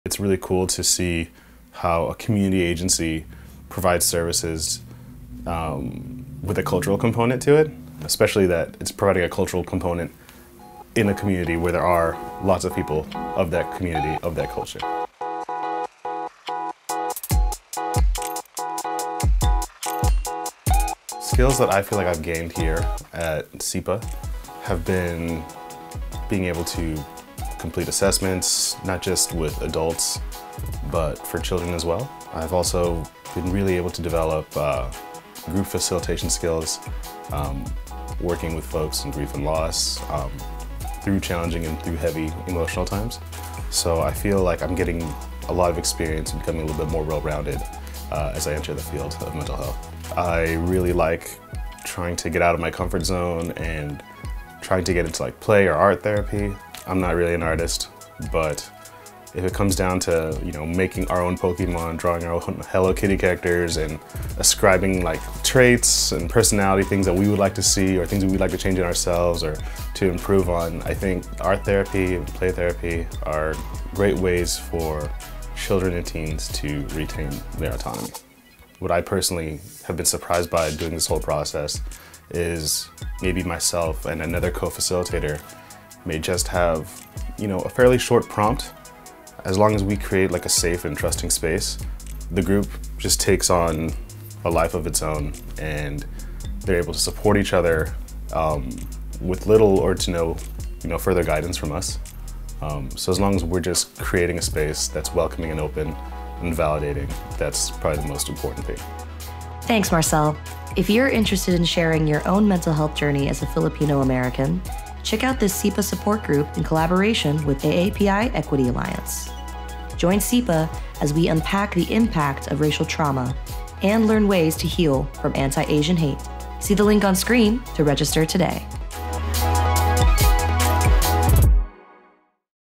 Meet Marcel (MH Intern) | SIPA Mental Health
“It’s really cool to see how a community agency provides services with a cultural component to it.”

And those services wouldn’t be possible without the staff and interns of our Mental Health Department!

We want to thank Marcel this NationalSocialWorkMonth for his incredible work with our youth clients as well as his dedication to our open Grief & Loss Support Groups. Maraming Salamat Marcel!

Got a question?  Fill out our General Inquiry Form